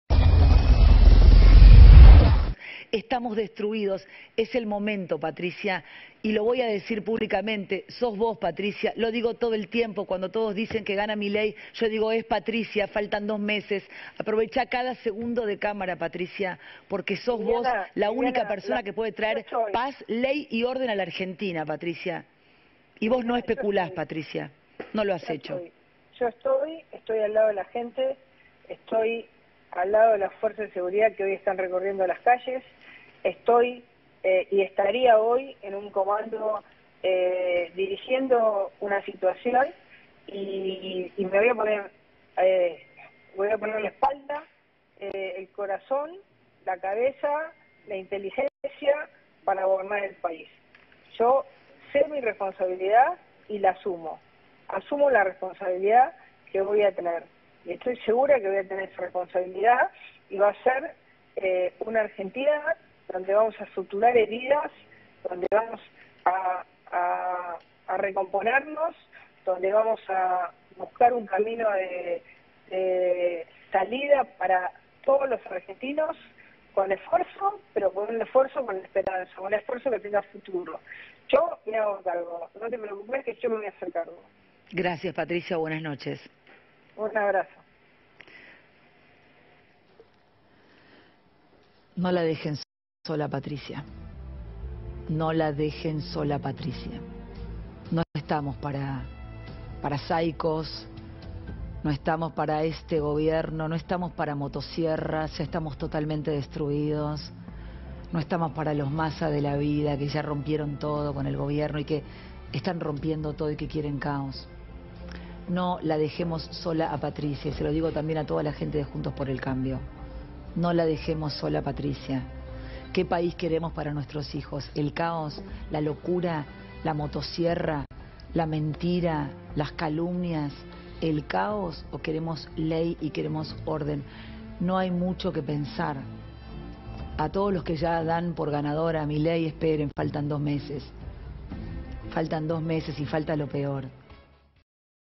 Viviana Canosa hace lo imposible por bajar a Milei
Canosa salio a bancar a Patricia Bullrich: "Vas a ganar vos, no Milei".